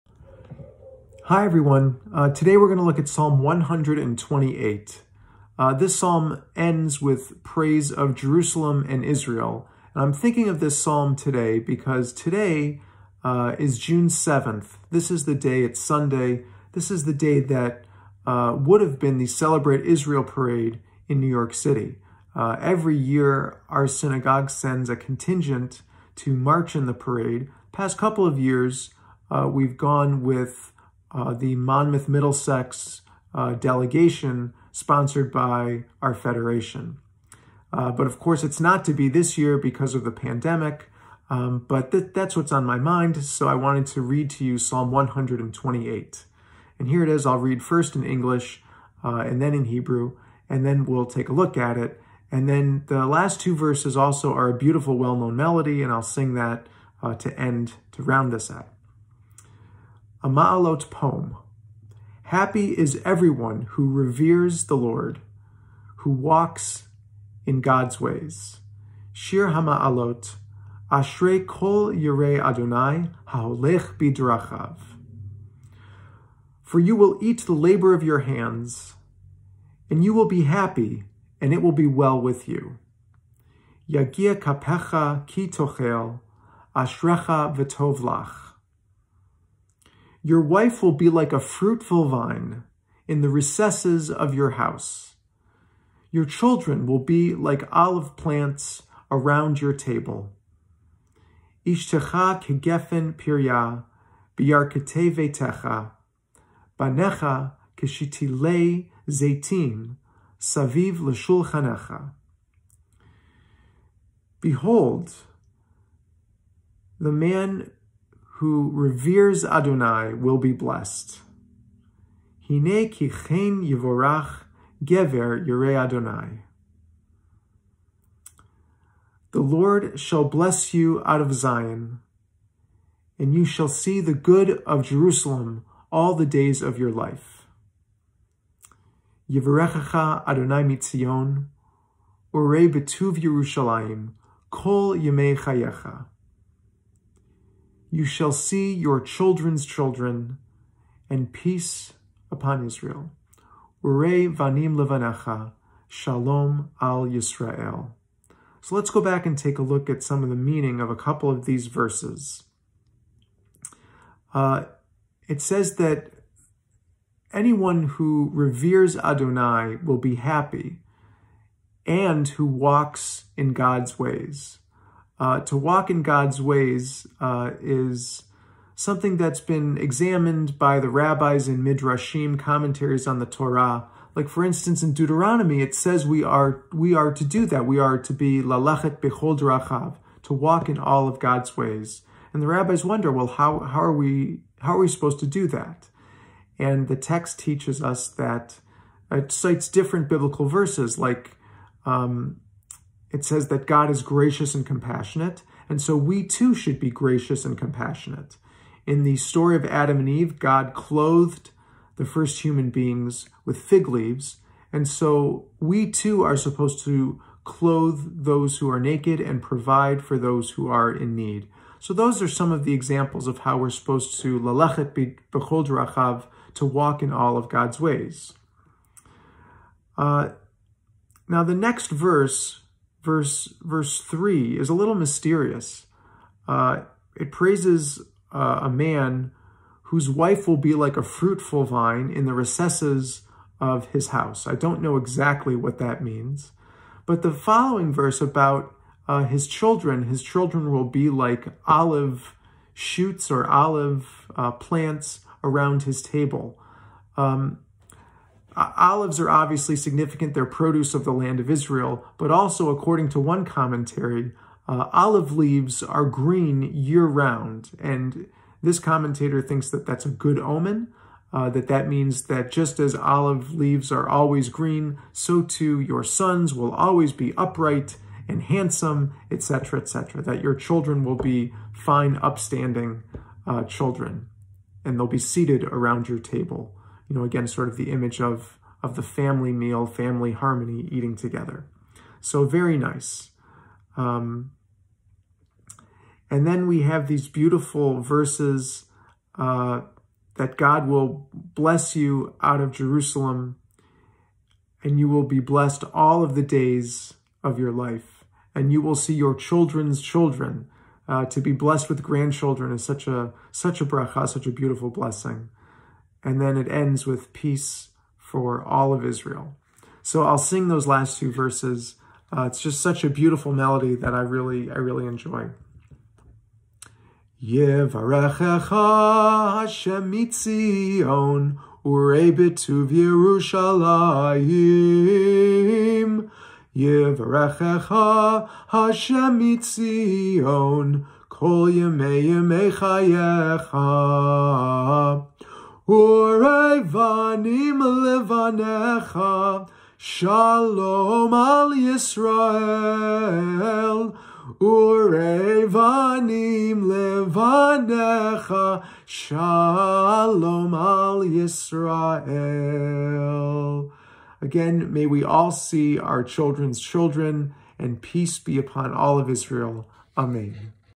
Psalm 128
The psalmist concludes with a call for peace for all of Israel.